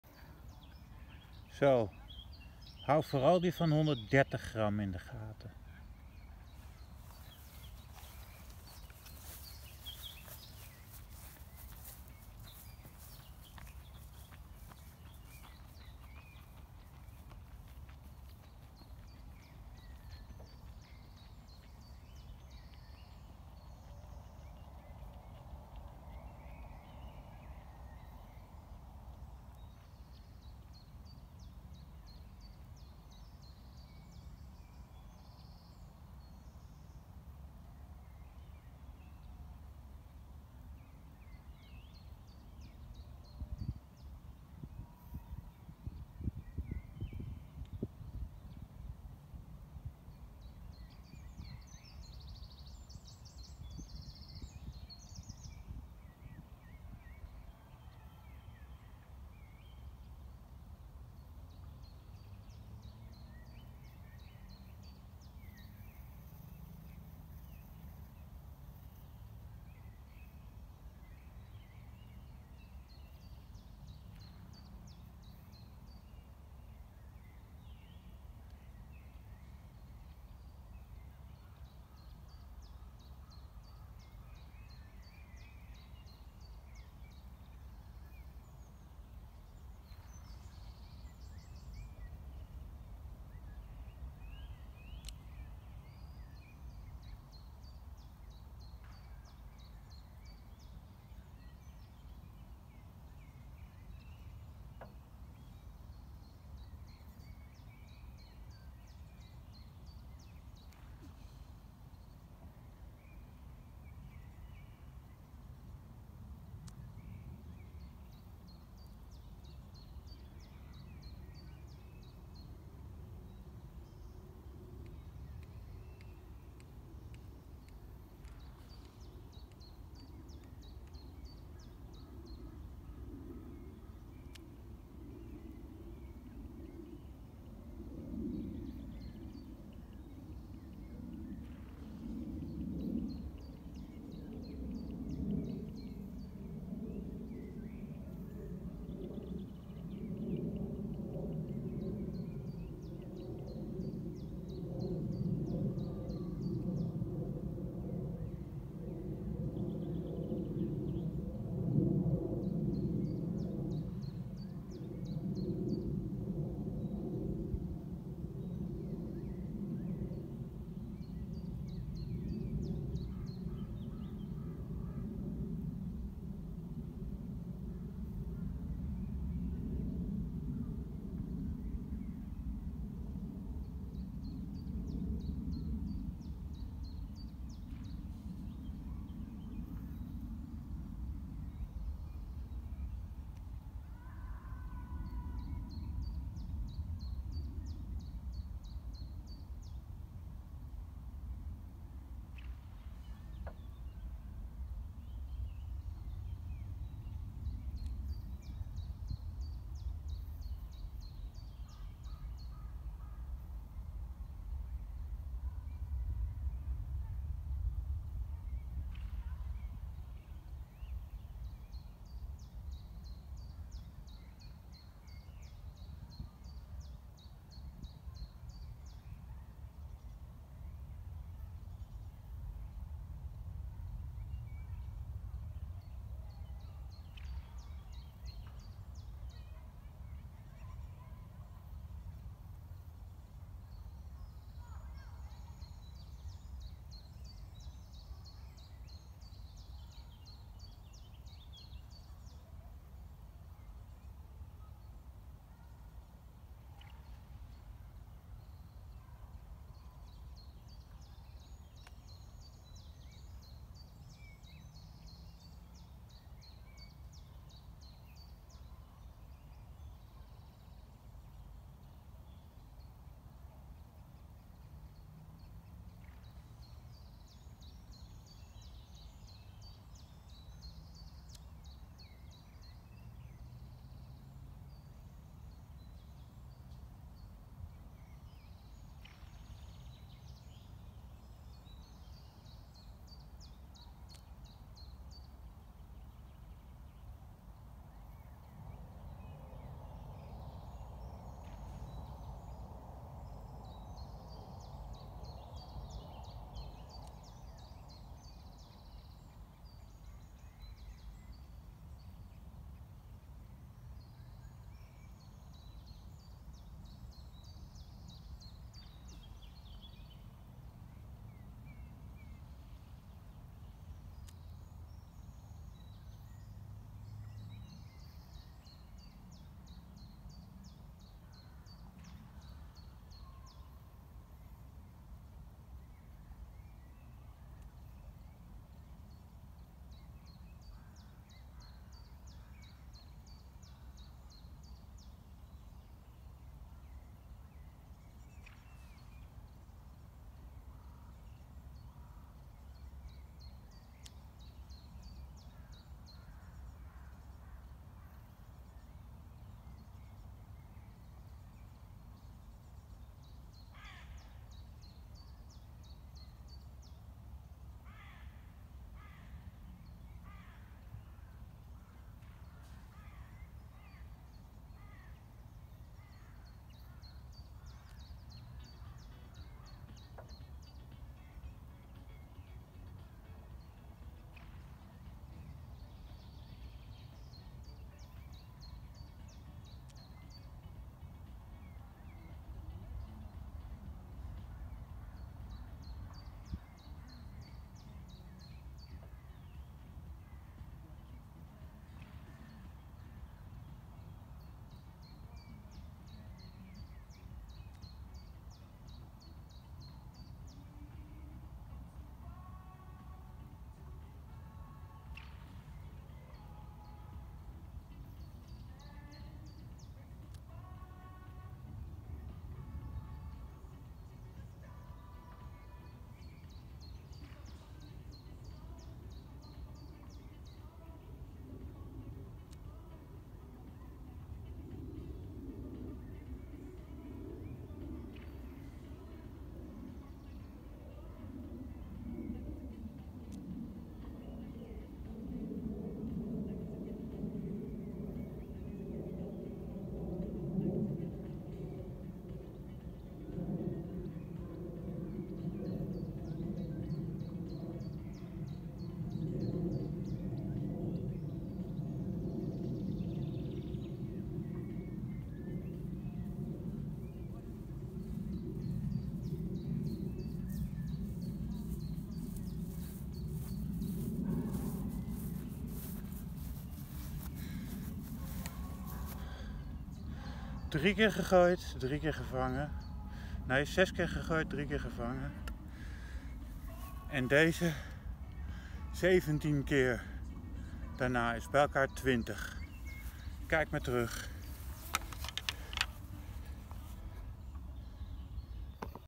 Boemerang gooi en vang 20 keer achter elkaar gevangen
Jaren lang geschuurd en geschaafd. Het resultaat wilde ik op het laatst zo dat de boemerang rustig terugkomt en dus makkelijk te vangen is. Dat is nu met de H1proP2 130 gram gelukt en dat kun je in dit filmpje zien ook: de eerste paar worpen doe ik met de H1P5, dat vertel ik erbij, dan doe ik die boemerang uit 1998 weg en pak ik de H1proP2 de worpen zijn hetzelfde maar de vangsten lijken zo makkelijk en zonder spektakel. Minder spectaculair dat wel maar ik ben nu in 2022 dus al wel 53 jaar oud, altijd in acht gehouden dat zo'n zware boemerang ook zo zijn gevaren heeft gooi ik er af en toe op los, nu dus makkelijker te vangen.